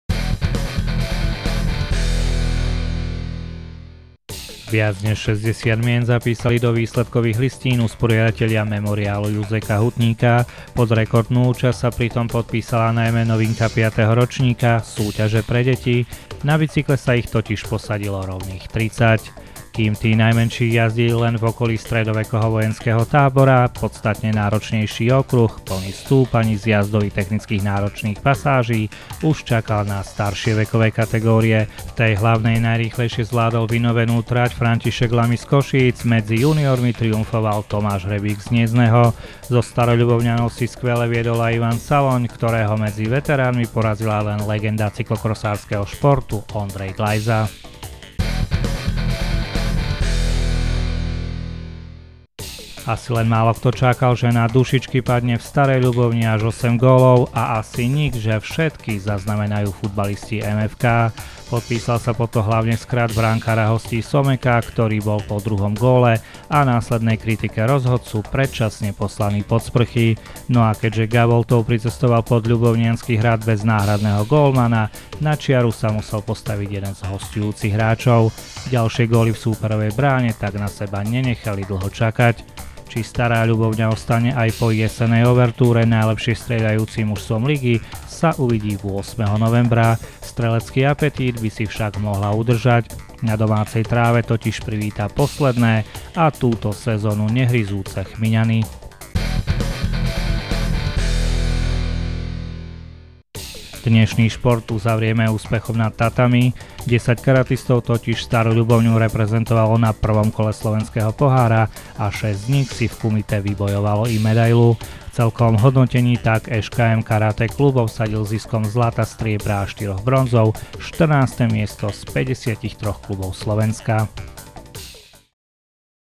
Športové správy
Doslova strelecký koncert MFK videla v nedeľu stovka divákov na futbalovom štadióne. Najskôr sa ale pozrieme pod Ľubovniansky hrad, kde sa deň predtým konal známy cyklokrosársky pretek.